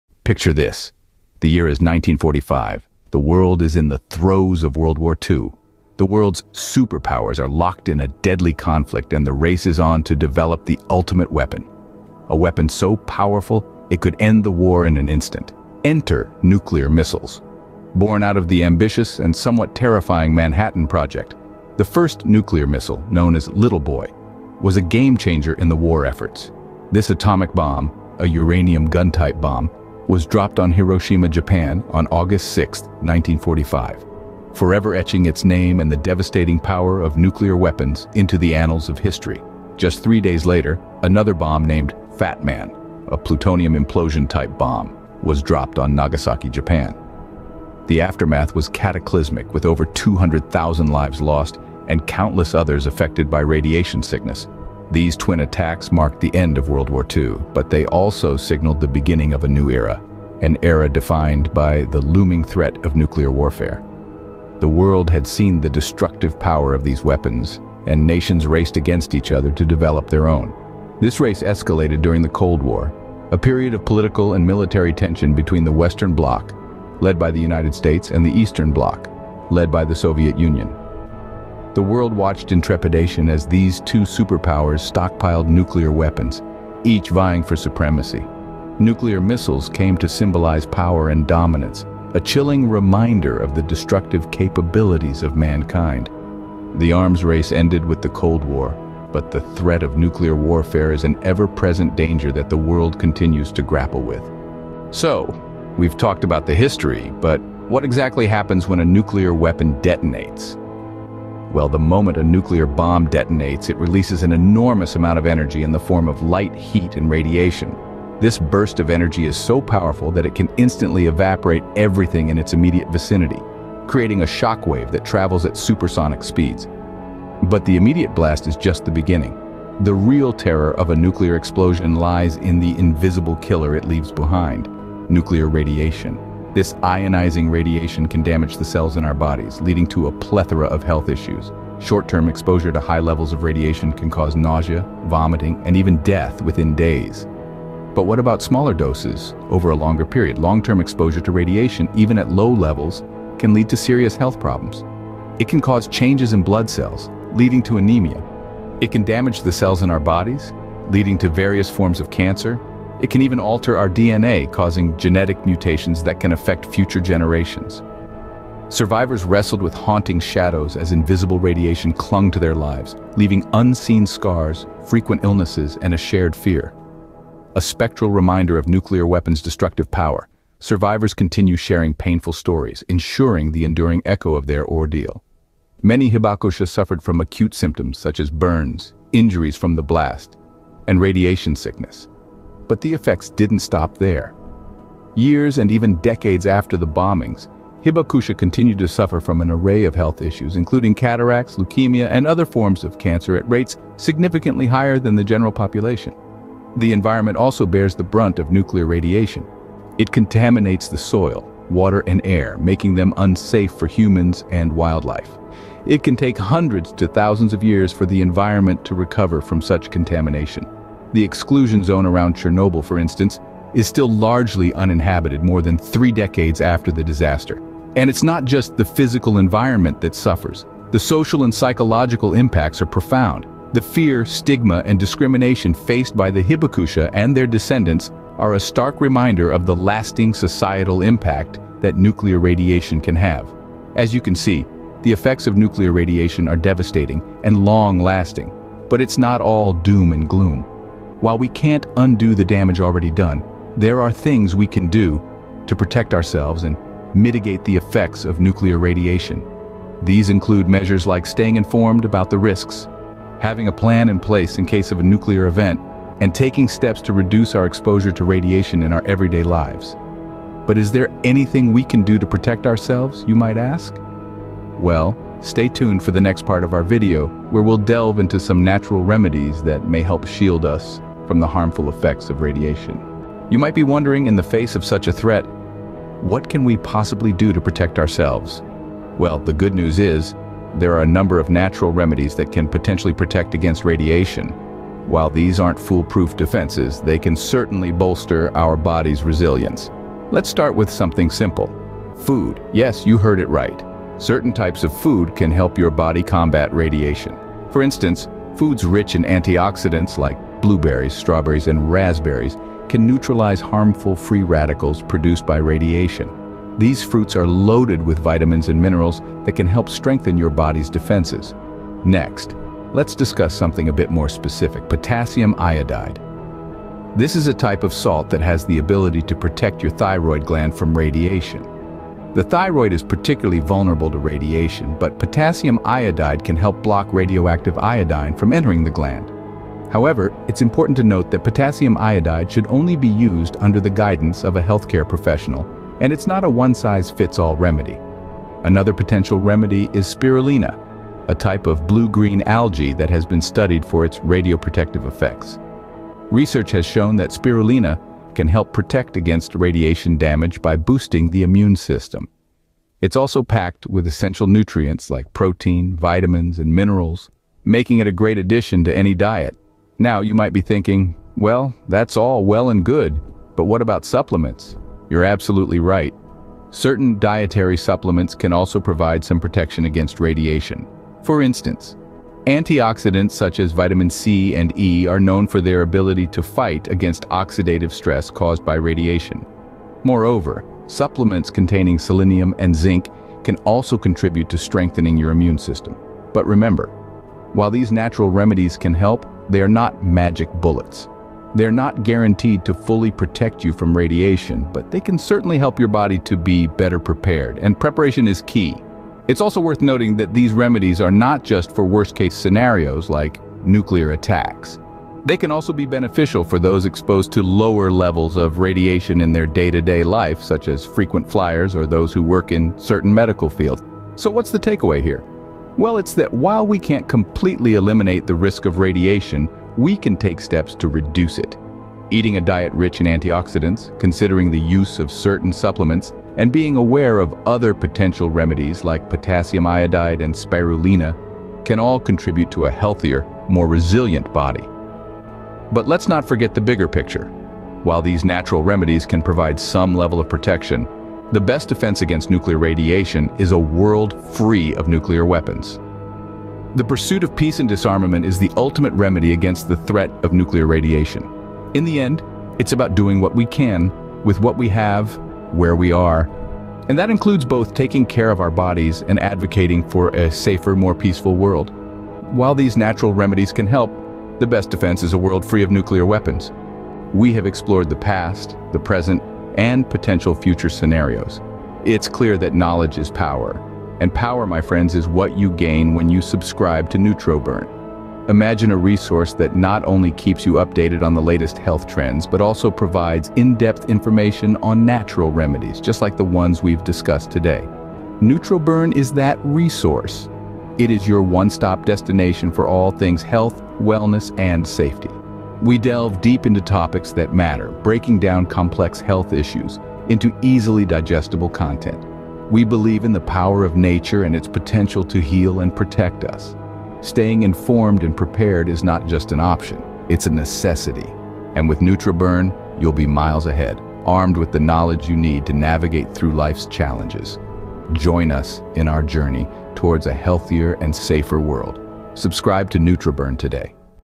This Plant Will Shield Against Radiation
Unlock the mysteries of nature's hidden shield against radiation in this eye-opening YouTube video. Dive into the clandestine world of a remarkable plant that holds the key to protection from harmful radiation. Explore its unique properties and learn how it has quietly become a guardian in the face of unseen threats. Join us on a journey of discovery as we unveil the secrets of this extraordinary plant and its potential impact on our well-being.